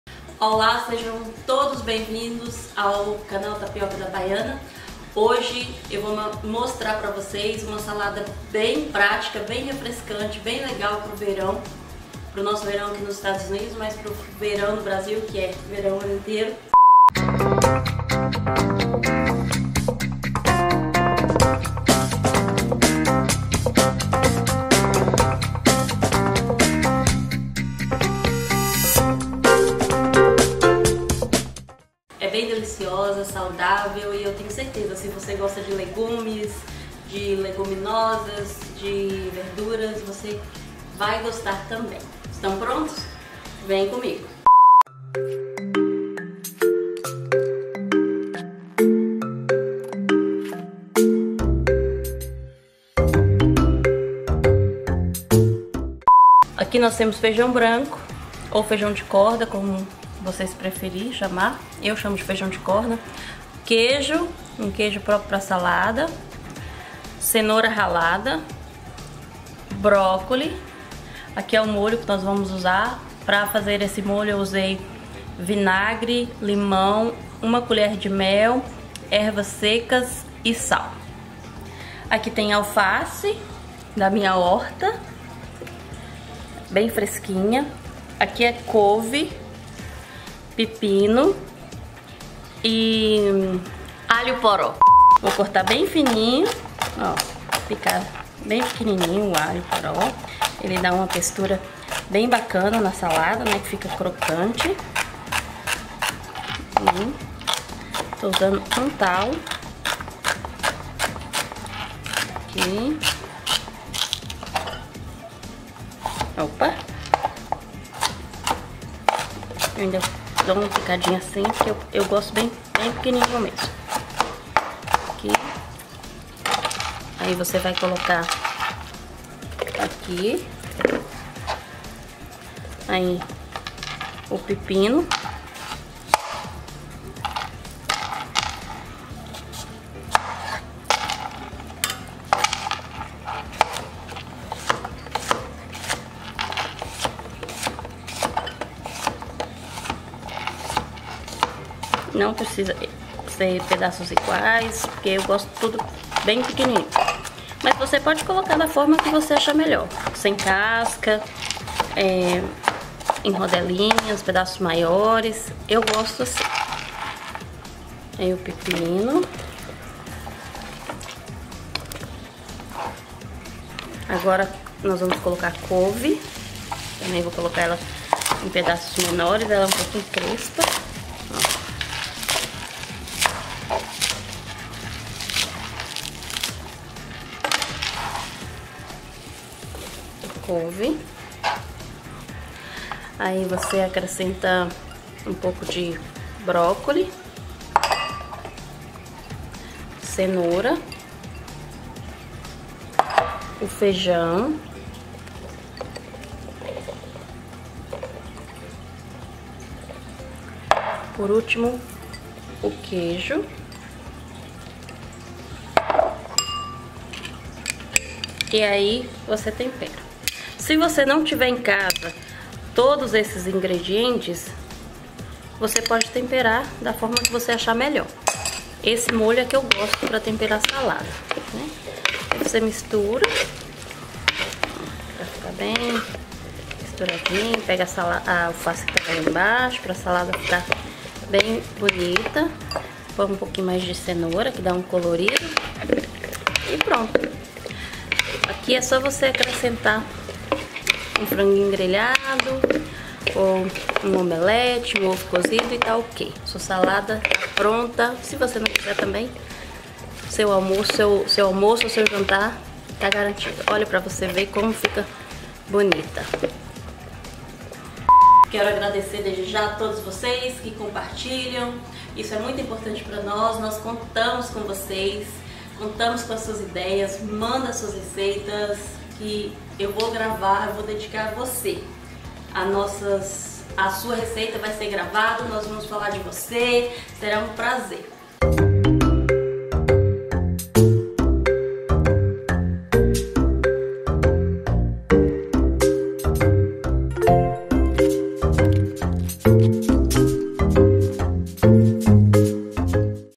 Tapioca da Baiana - Salada (Verão)
Aqui nos Estados Unidos é temporada de calor e por isso gostaria de compartilhar algumas receitas de salada que faço para minha família nessa época.